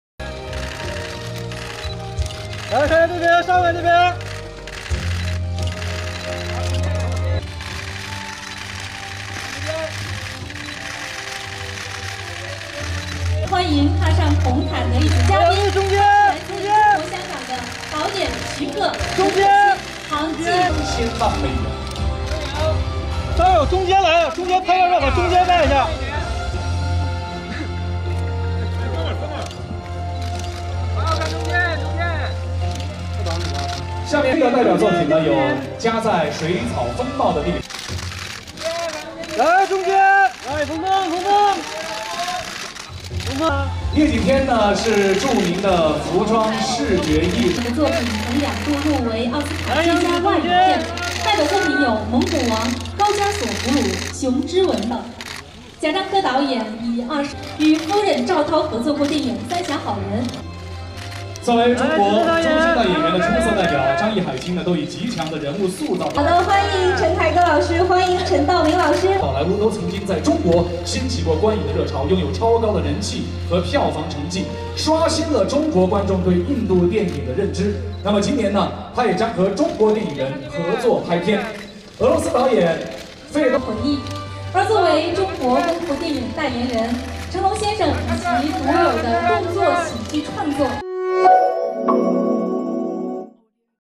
章子怡成龙陈道明等亮相亚洲影视周红毯
5月16日，亚洲文明对话大会期间，“亚洲影视周”启动仪式在北京举行。成龙、章子怡、陈道明、陈凯歌、杨幂、黄晓明、陈坤、邓超、蔡徐坤等人走上红毯，星光熠熠。此外，印度导演、演员阿尔米·汗，日本编剧、导演山田洋次，越南法裔导演陈英雄等亚洲知名影人也走上红毯，共话亚洲电影。（张子胥）